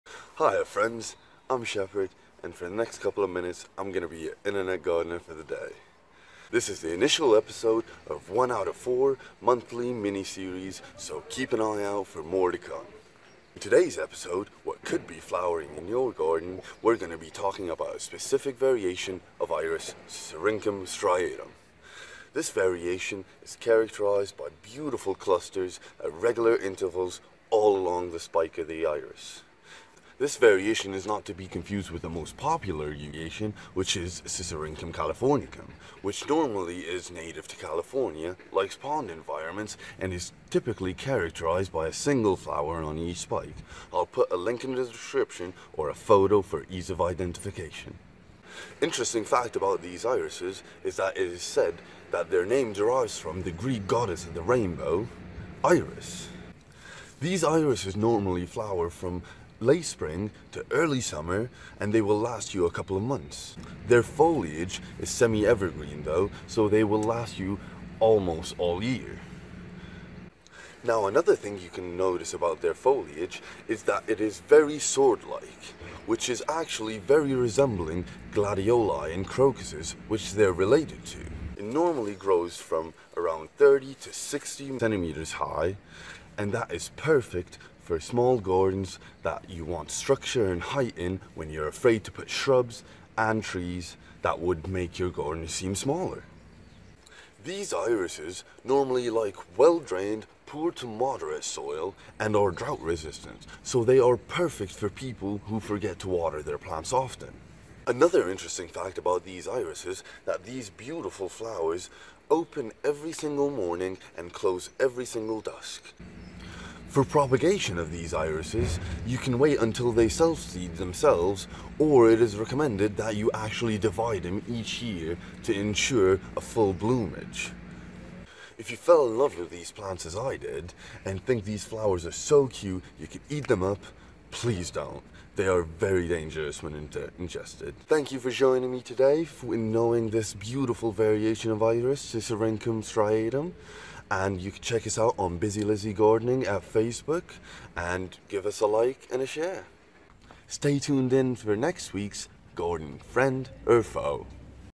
What could be flowering in your garden. Iris (Sisyrinchium Striatum) E1
This is 1 out of 4 monthly gardening series, where we will be exploring different aspects of garden care from planting, growing vegetables, creating structures in your garden to knowing plants and animals that are beneficial and what might be harmful for your garden.

We are local gardeners in Cambridge, UK. You can check us out on our facebook page at Bizzie Lizzie Gardening: